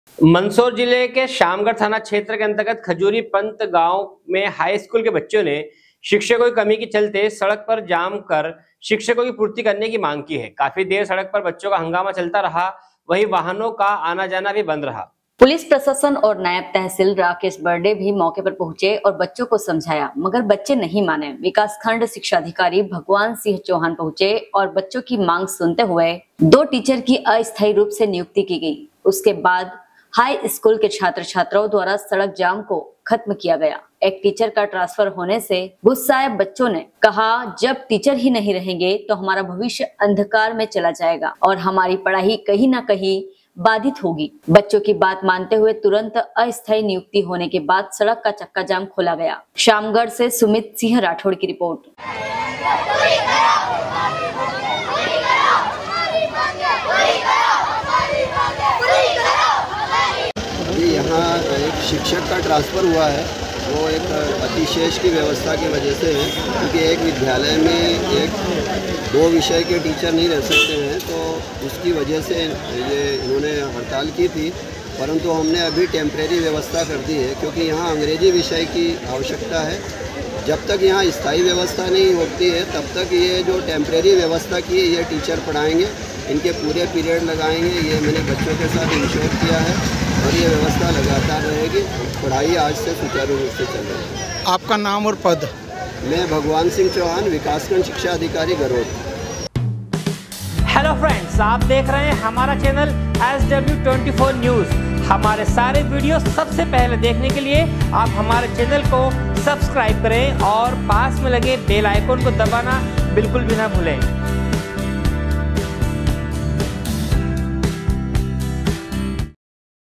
बच्चों का सड़क जाम, शिक्षकों की कमी पर उठी आवाज, दो टीचर की अस्थाई नियुक्ति
हाई स्कूल छात्रों का प्रदर्शन: शिक्षकों की कमी दूर करने की मांग पर हुआ सड़क जाम
खजूरी पंत गांव में हाई स्कूल के छात्रों ने शिक्षकों की कमी के खिलाफ सड़क जाम कर विरोध जताया। हंगामा बढ़ने पर विकासखंड शिक्षा अधिकारी ने छात्रों की मांग पर दो अस्थाई टीचर नियुक्त किए। छात्रों ने कहा, “टीचर नहीं होंगे तो भविष्य अंधकार में जाएगा।” नियुक्ति के बाद सड़क का जाम समाप्त हुआ। शामगढ से सुमित सिंह राठौड़ कि रिपोर्ट